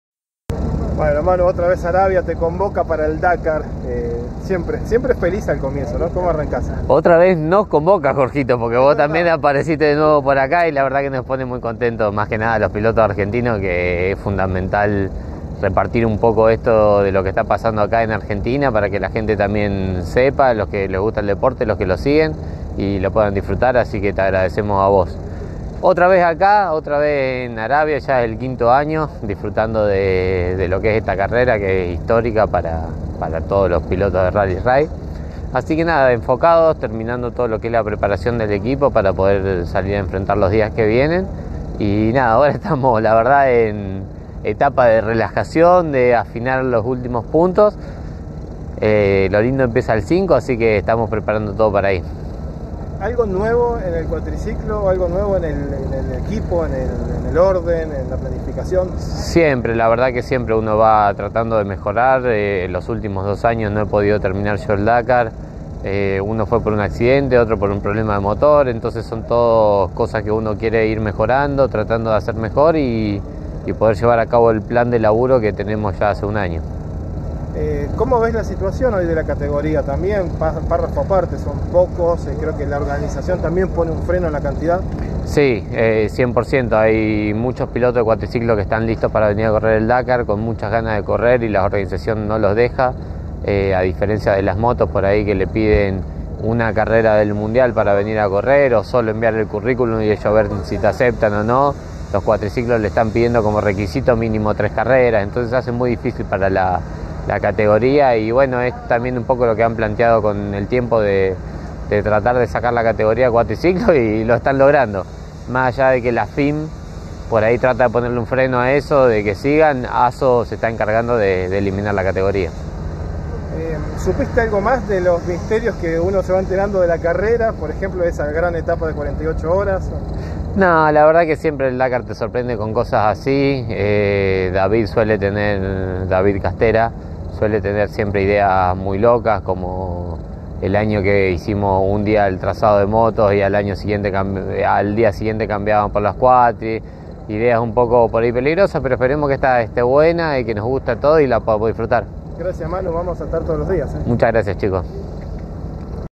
Manuel Andújar defiende la continuidad de los Quads en el Dakar
El campeón 2021 en cuatriciclos dialogó con Campeones y contó cómo percibe la actualidad de la categoría.